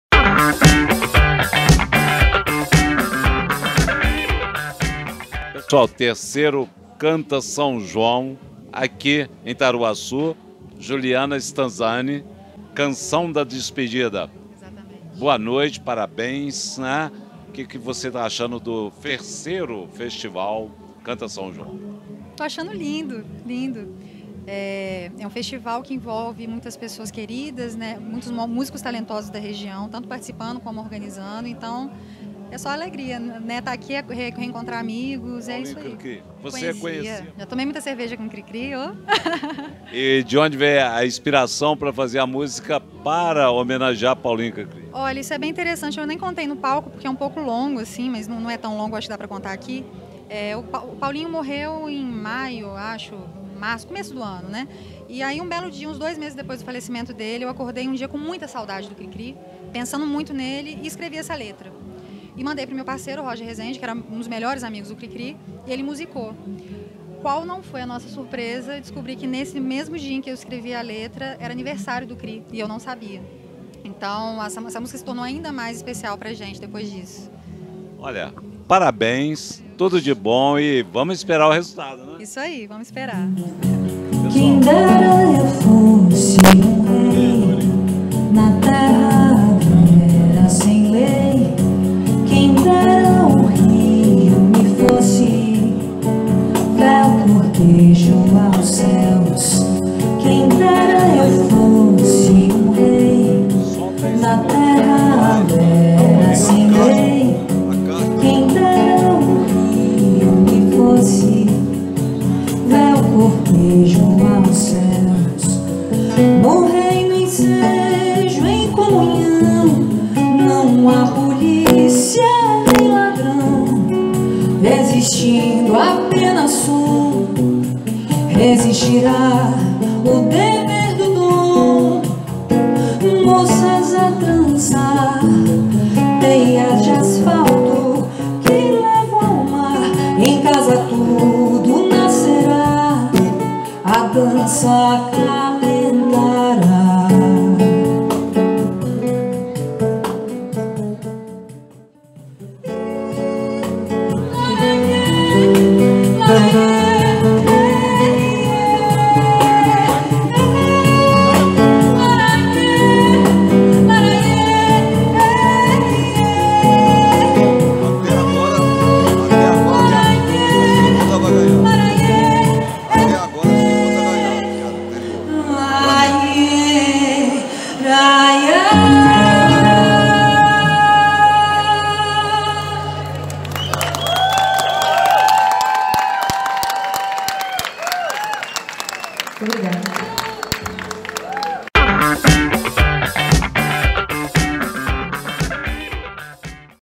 Paulo Azevedo entrevista Juliana Stanzani no 3º Canta São João
Paulo Azevedo entrevista Juliana Stanzani durante o  3º Canta São João no distrito de Taruaçu emJoão Bosco, festival de canções inéditas e tem como troféu  Paulinho Cri que foi realizado no distrito de Taruaçu,  nos dias 12 e 13 de julho de 2019. nesta edição, contou com show de João Bosco, ícone da Música Popular Brasileira (MPB), na noite de sábado (13/7), depois das apresentações das canções finalistas.